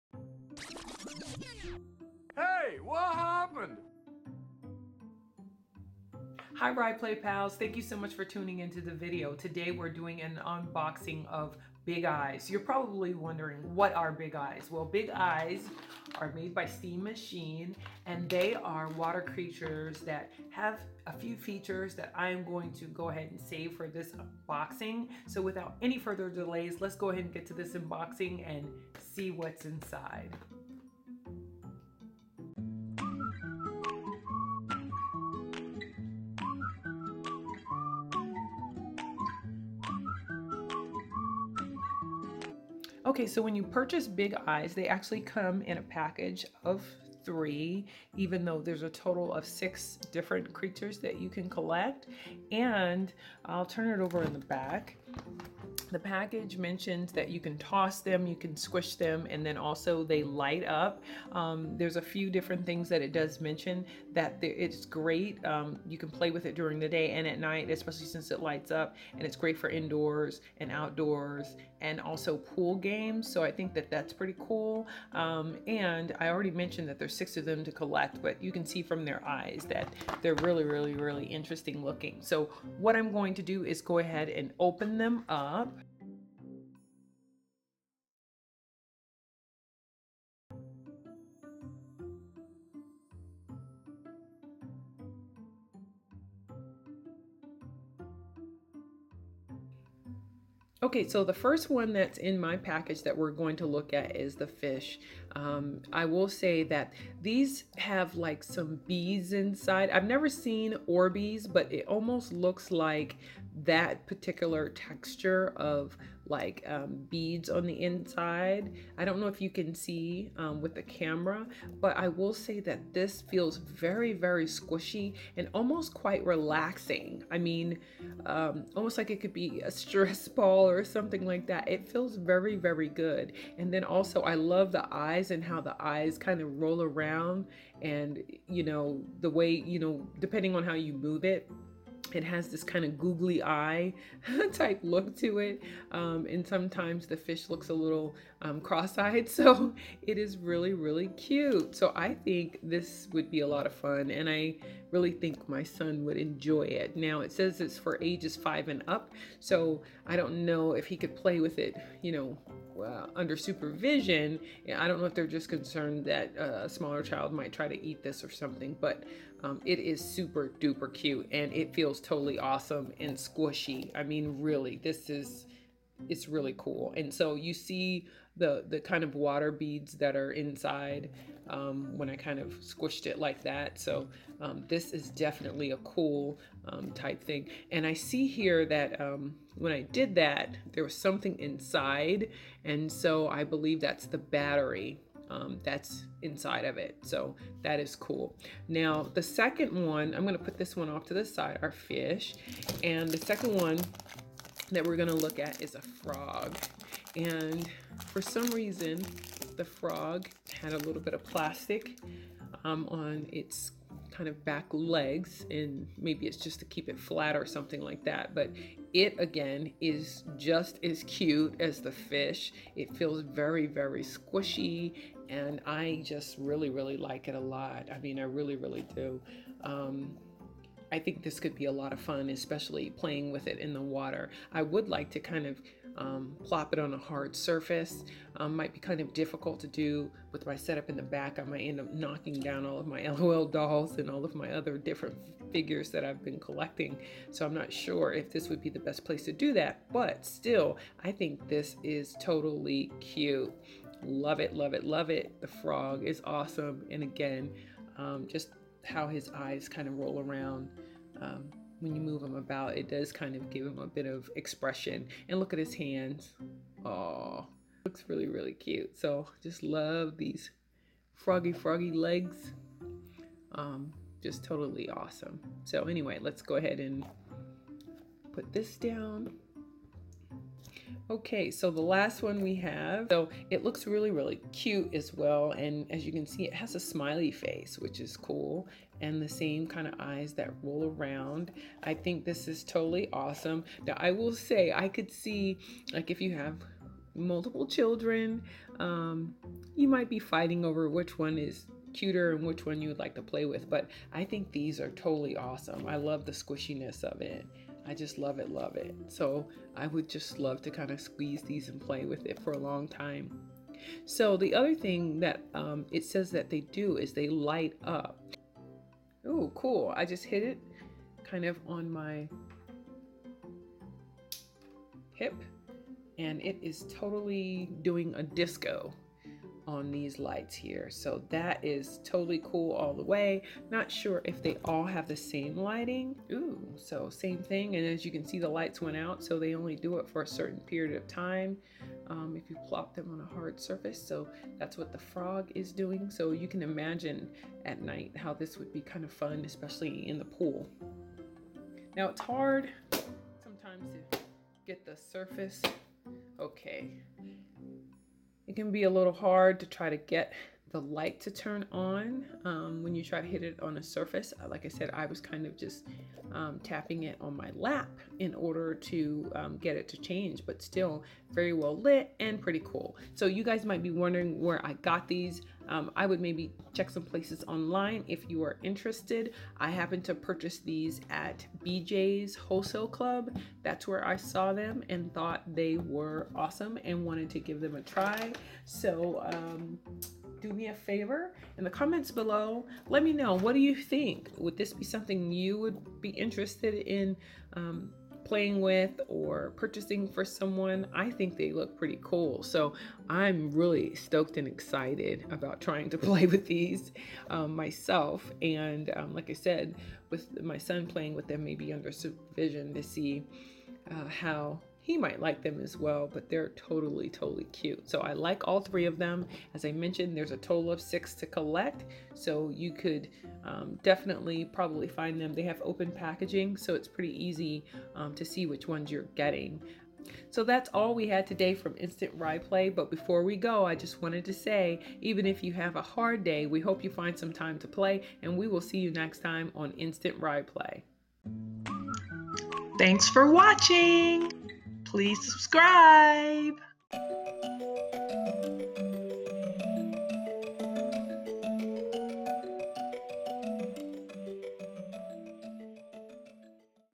UNBOXING OF "BIG EYES" POOL TOYS
Big Eyes by Stream Machine are squishy water creatures that you can you can squish, toss, and play with day or night because they light up! Watch as we unbox these cute squishy creatures and let you know first-hand what we think about them!

And remember, even if you had a hard day, we hope you find some time to play!

Mailing Address:

Instant Ry Play
400 West Peachtree Street NW